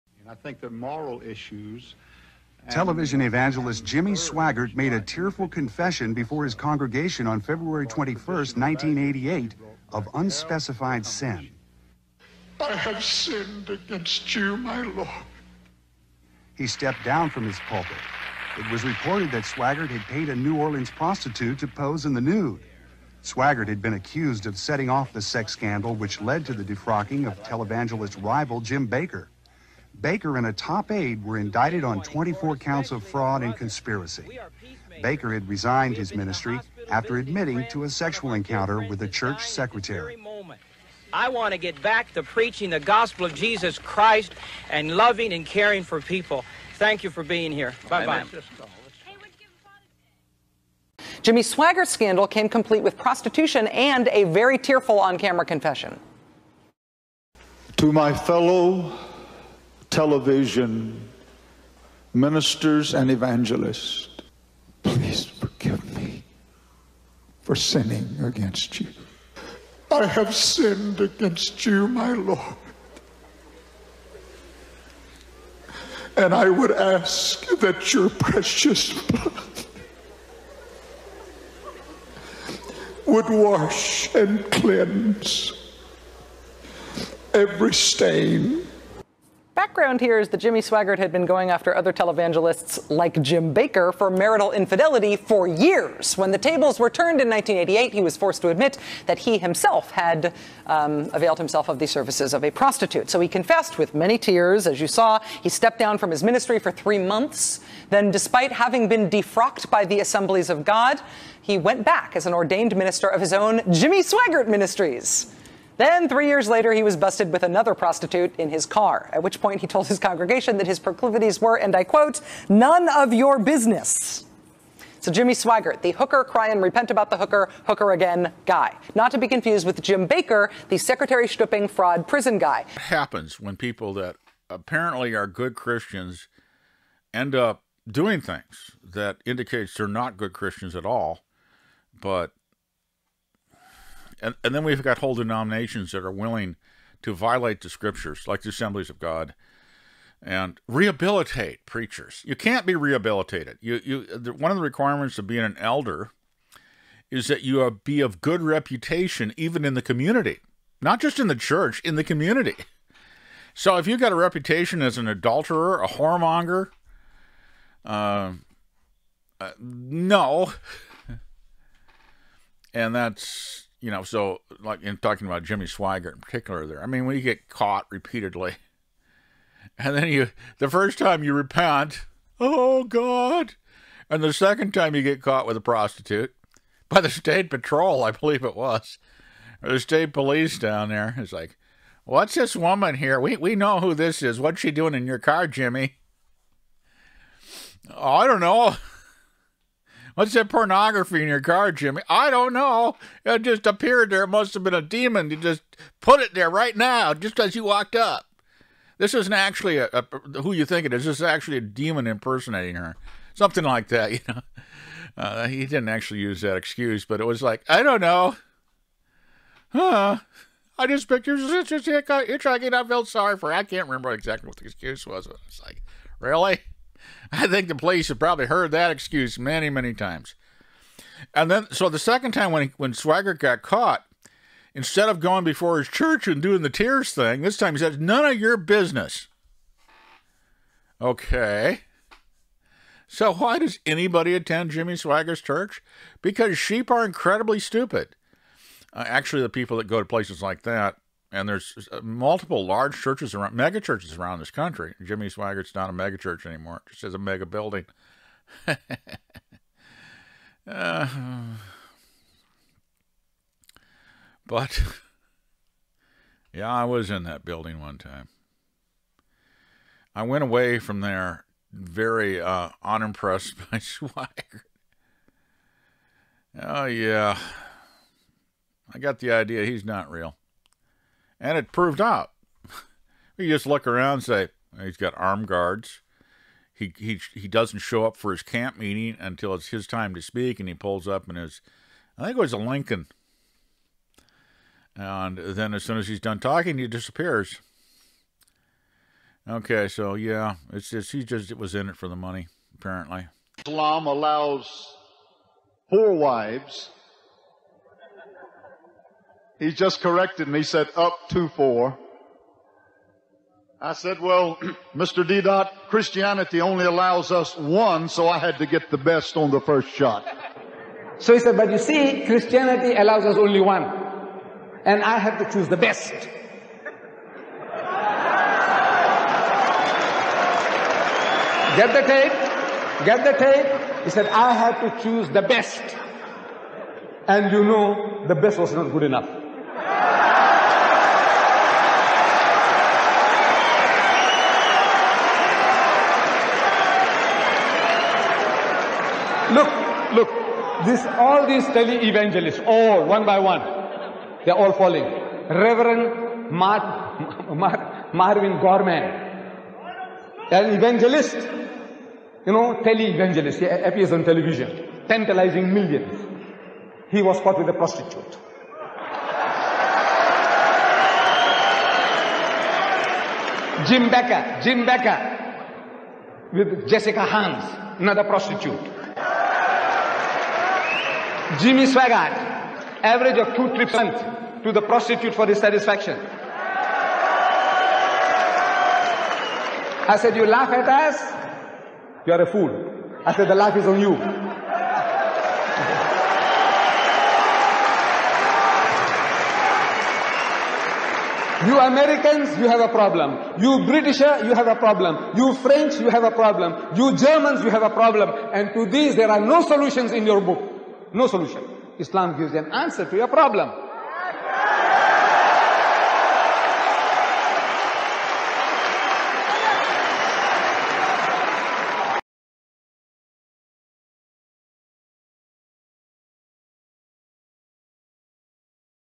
Christian preacher Stephen Anderson explains why he left Jimmy Swaggart's Megachurch!
A country preacher's take on the Scriptures, Christianity and the World